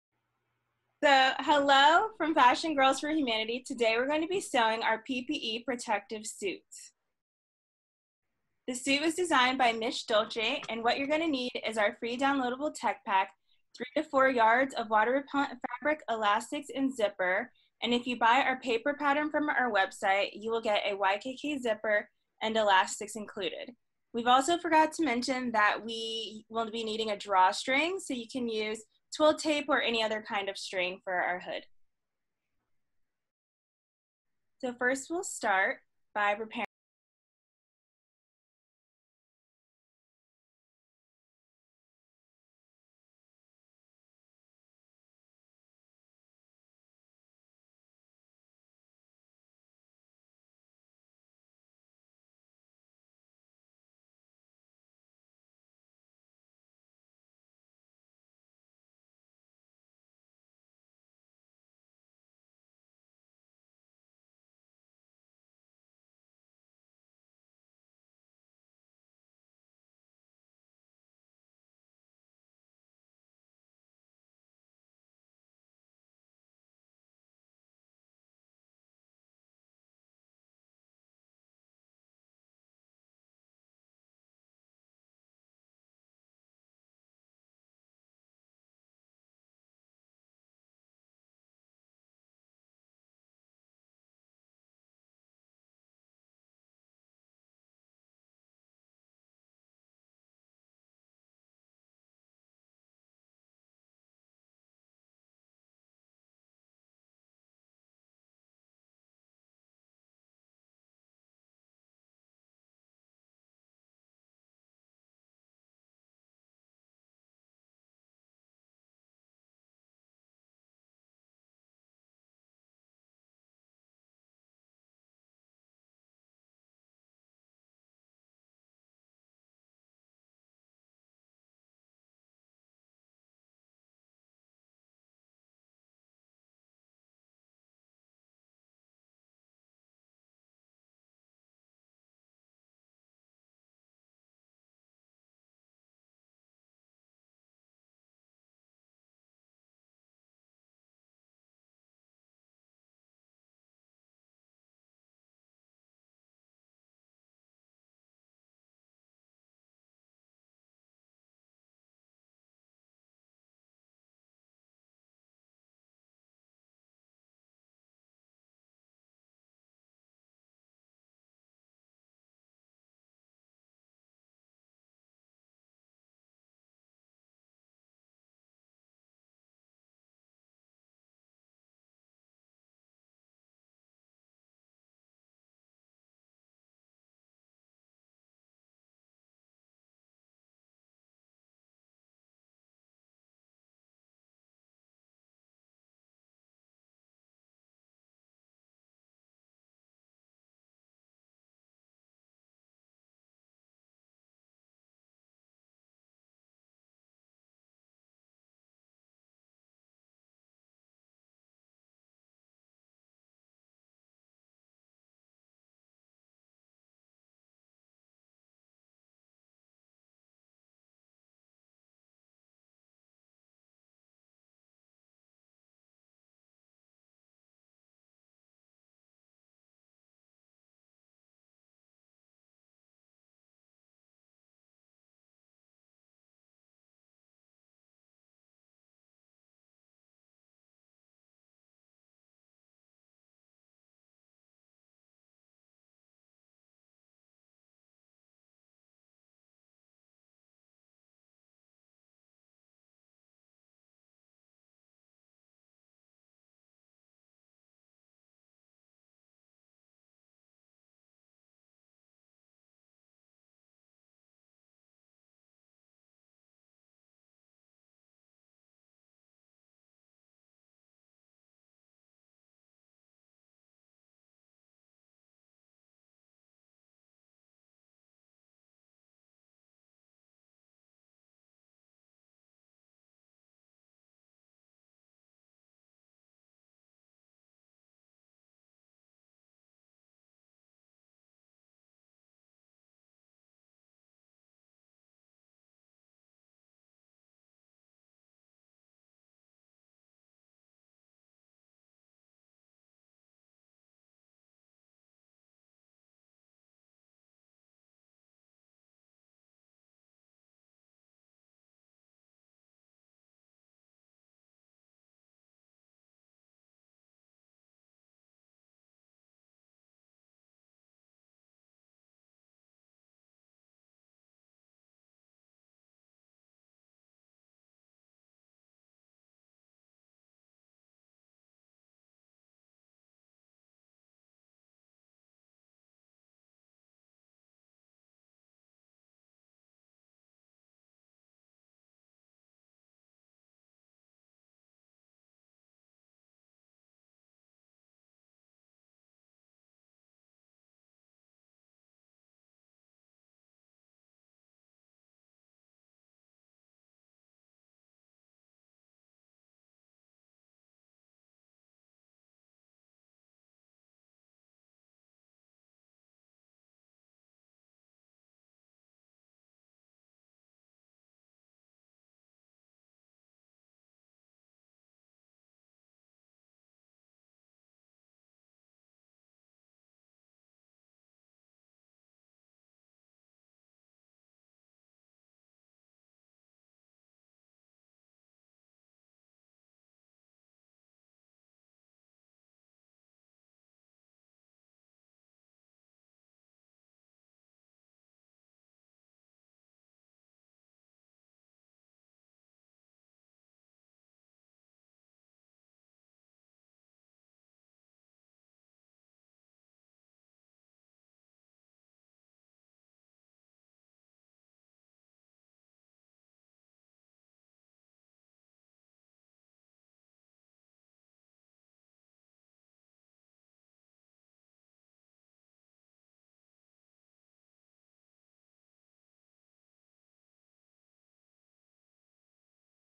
Como coser un traje de proteccion EPI
Este video trata sobre: ​​DIY "Fácil" CÓMO COSER UN TRAJE PROTECTOR PPE PARA COMBATIR EL COVID-19

El patrón descargable gratuito está disponible en Fashion Girls for Humanity.
Una vez que hagas tus propios vestidos, etiqueta a @FashionGirlsforHumanity a través de Facebook e Instagram para compartir tu historia.

En 4 meses, más de 170,000 personas de 170 países descargaron patrones y vieron este breve tutorial sobre cómo hacer una bata de aislamiento de Fashion Girls for Humanity la organización sin fines de lucro 100% voluntaria de ayuda en casos de desastre. comenzó hace 9 años. Convertí nuestra especificación al sistema métrico en respuesta a una solicitud de la Fundación Henry M. Jackson para el Avance de la Medicina Militar en Uganda, y descubrí que un equipo de costureras en Idaho compartía el patrón y fabricaba PPE para la Residencia de Medicina Familiar de Idaho. Ahora incluimos información sobre vestidos para un tamaño más grande, patrones para material Tyvek u otra tela no tejida. También contamos con recursos sobre tela, materiales precortados y adornos para su producción.

Estoy agradecido e inspirado por cada uno de ustedes que ha enfrentado el desafío y ha superado esta crisis de salud sin precedentes de nuestro tiempo. Hay muchas cosas que todos podemos hacer desde casa, como compartir nuestro aprendizaje y ofrecer habilidades para generar un impacto. Como emprendedora y fundadora de una organización sin fines de lucro, creo que la moda puede tener un impacto social, incluso salvar vidas o ayudar a los trabajadores médicos de primera línea a salvar vidas, y es nuestro momento de hacer todo lo que podamos. ¡Gracias!

Estamos en esto juntos. getmeppe # vestidos makeimpact # máscaras

Materiales:

En el video usé la tela disponible, pero mira la tela Ripstop de:

A partir de abril de 2020, los CDC recomiendan lo siguiente

Capacidad de contingencia: las medidas pueden cambiar las prácticas estándar diarias, pero es posible que no tengan un impacto significativo en la atención brindada al paciente o la seguridad del personal de salud (HCP). Estas prácticas pueden usarse temporalmente durante períodos de escasez esperada de batas de aislamiento.

¿Cualquier pregunta? ¿Mejora? Comentarios en los comentarios por favor
Sigue Instagram y etiqueta tu vestido final a @fashiongirlsforhumanity